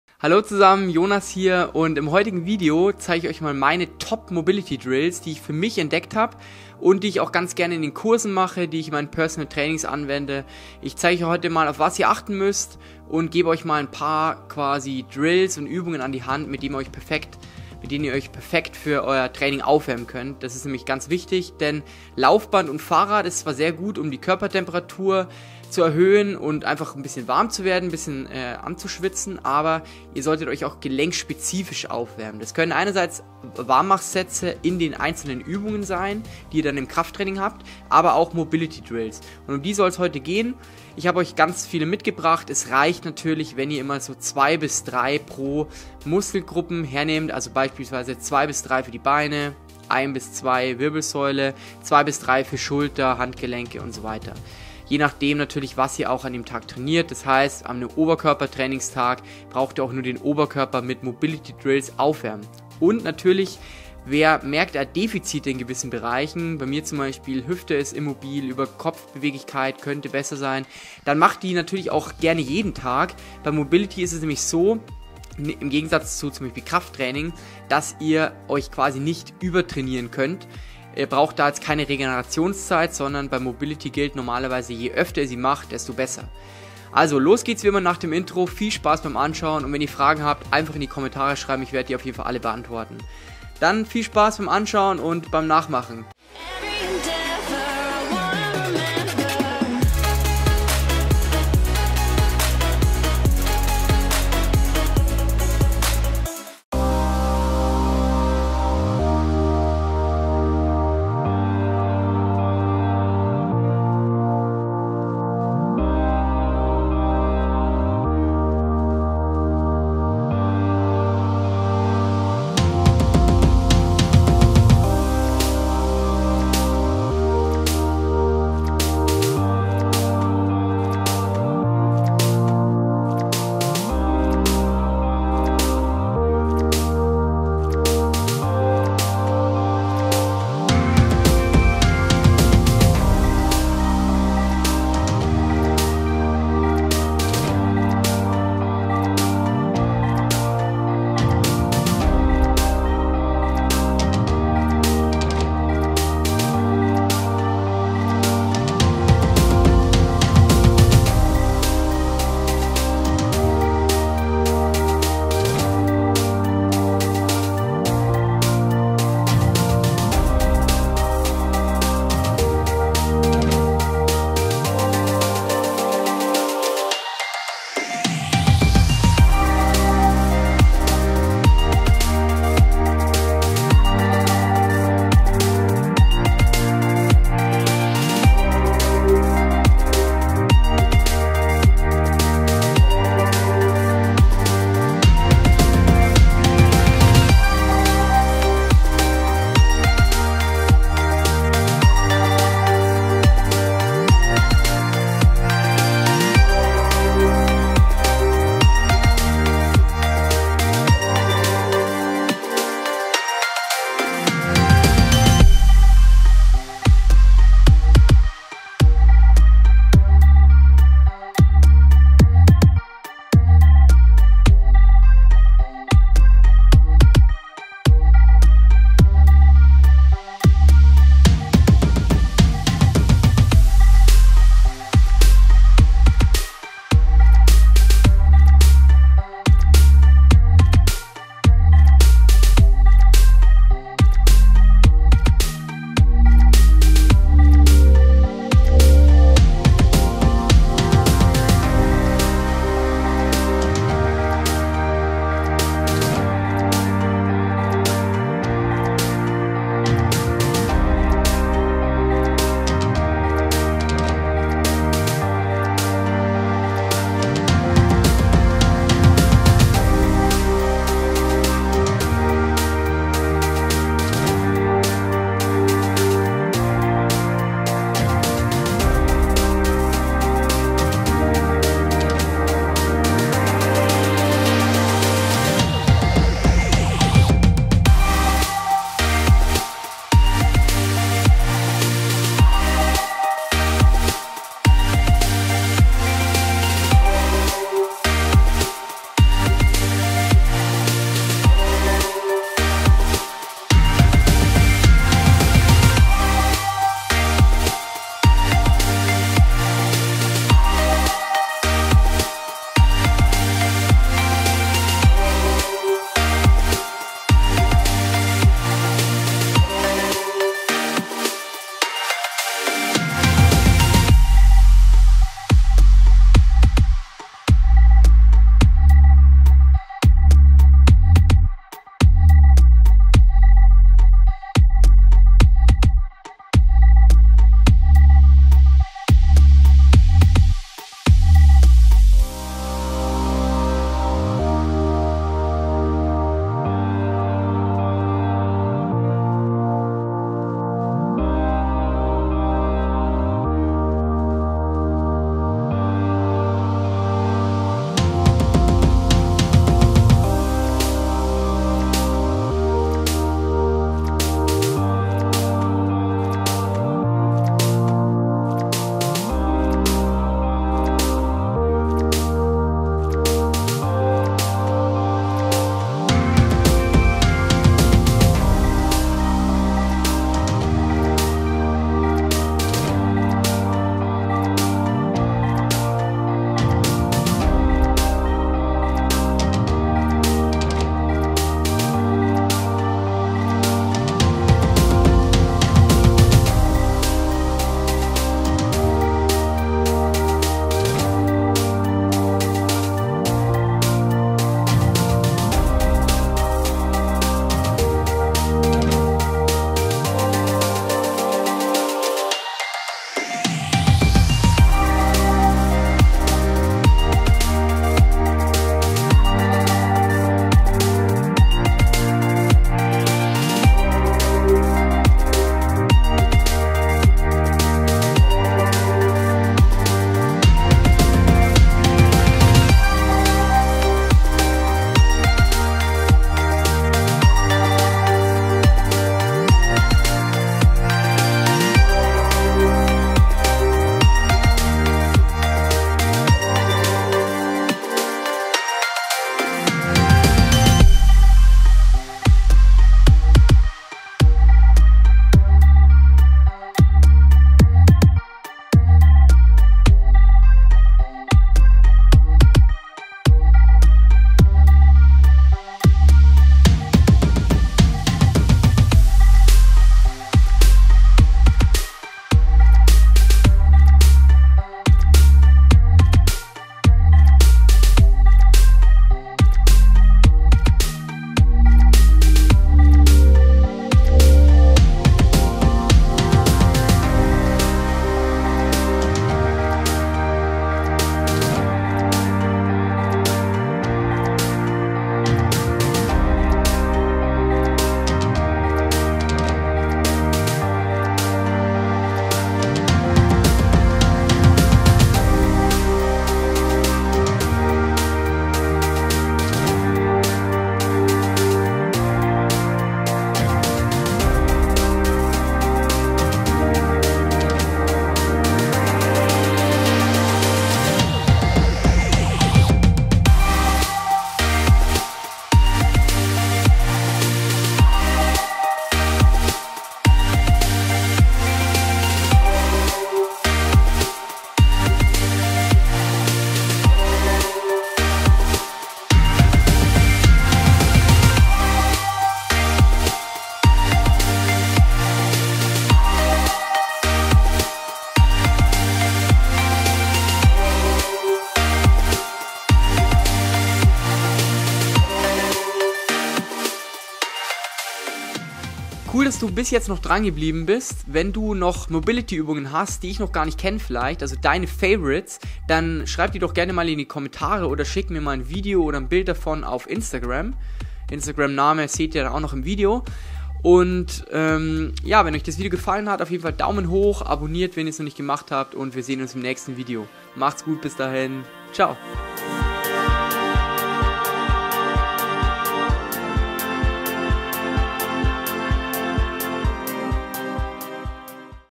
Die besten Mobility Übungen für das Krafttraining | Warm-Up Routine Tutorial
Das braucht jeder Kraftsportler

Du kaufst öfters bei Amazon ein? Nutze diesen Link bei deinen Einkäufen um uns OHNE Mehrkosten zu unterstützen:

Jonas' Kamera
Jonas' Objektiv
Felix' Kamera

by Jonas Schulze & Felix Köll